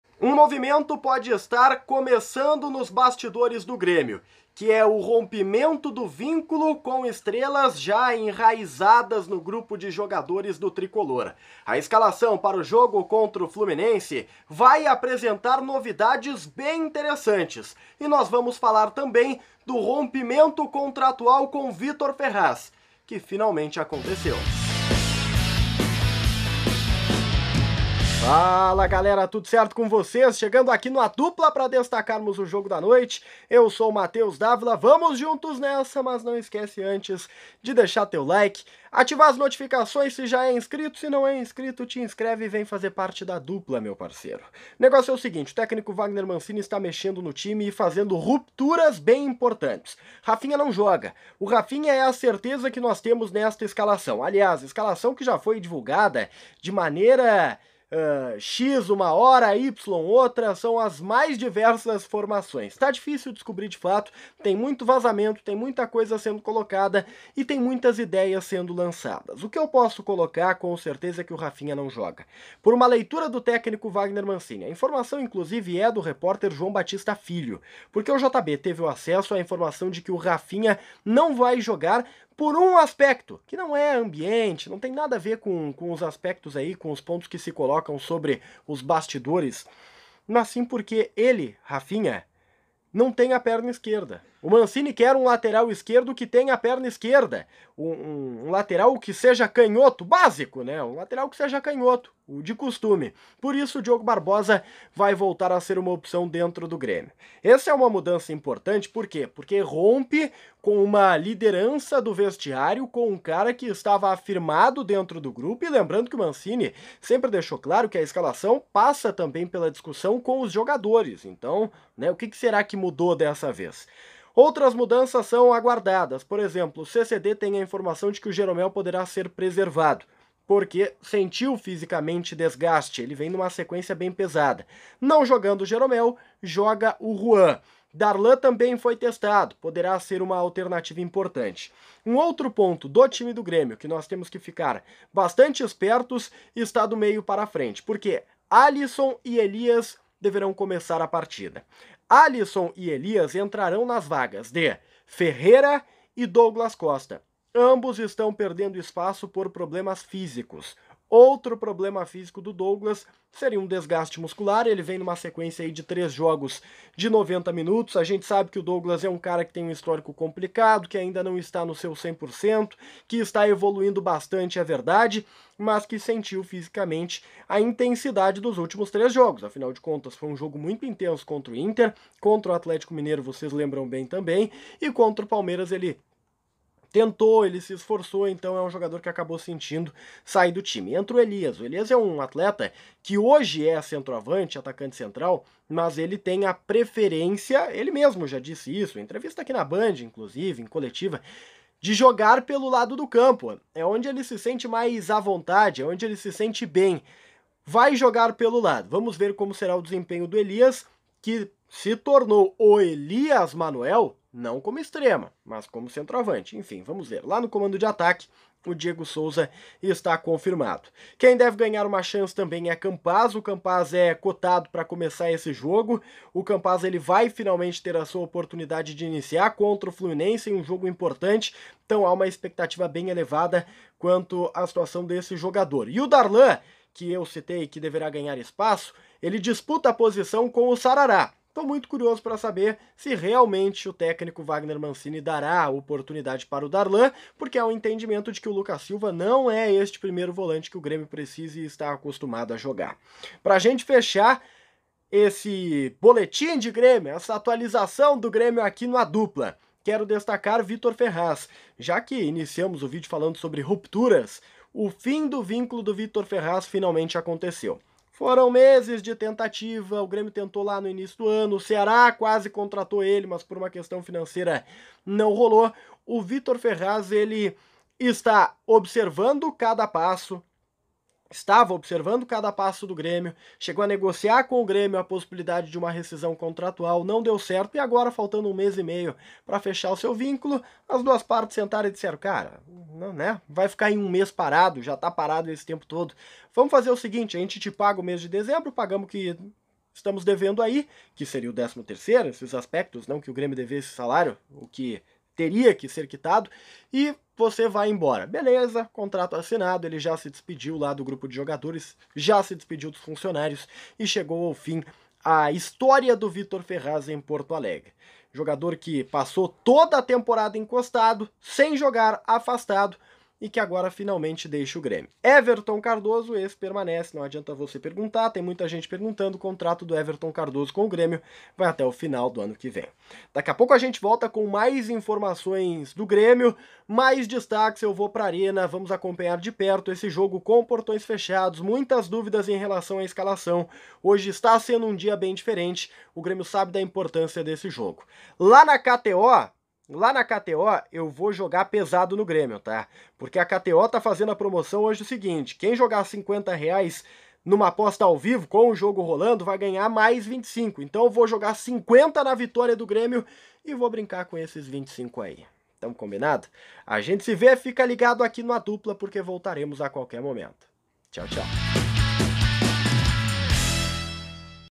🔵⚫ Grêmio: Cascudos perdendo espaço? | Adeus a Victor Ferraz | Time contra o Fluminense
Às 21h30 tem decisão contra o Fluminense!

- Segue o D'Avila no Insta: @davilamatheus